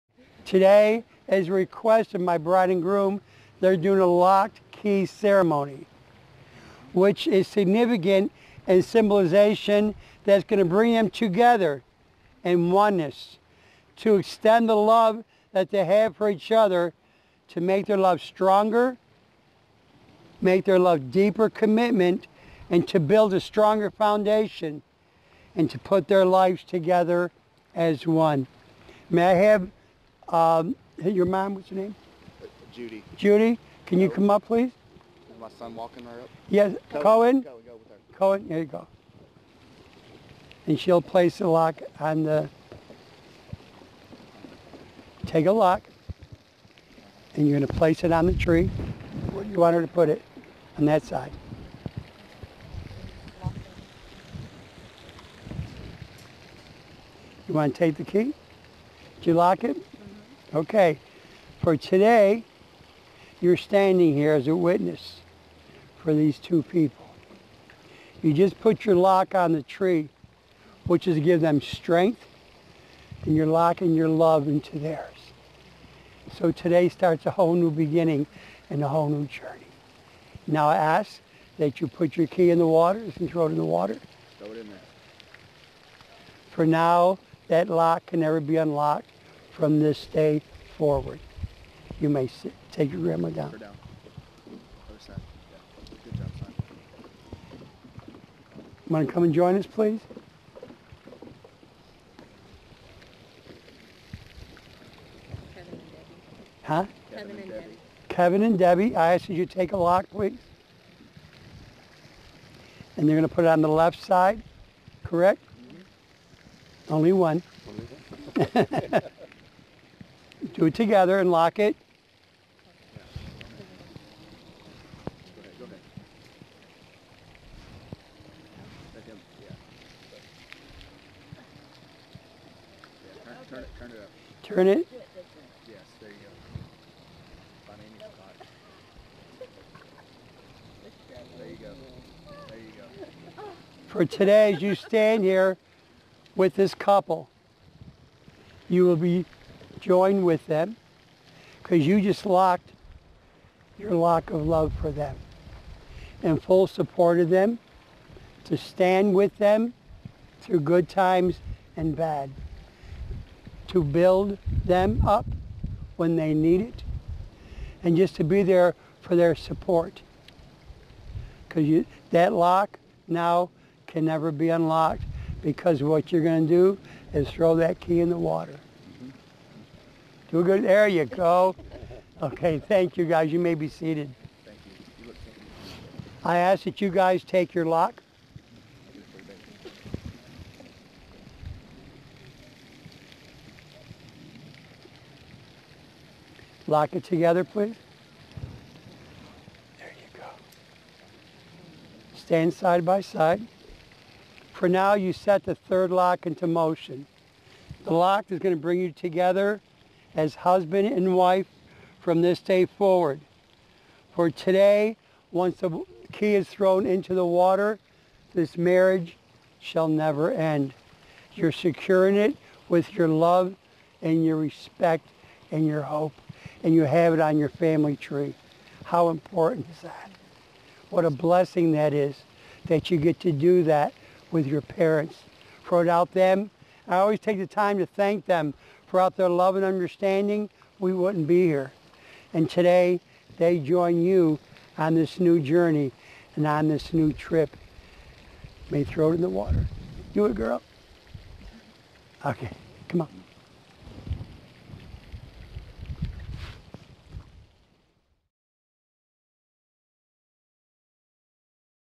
Key and Lock Ceremony at Kemah Gardens
Key and Lock ceremony conducted at Kemah Gardens on November 19th, 2017.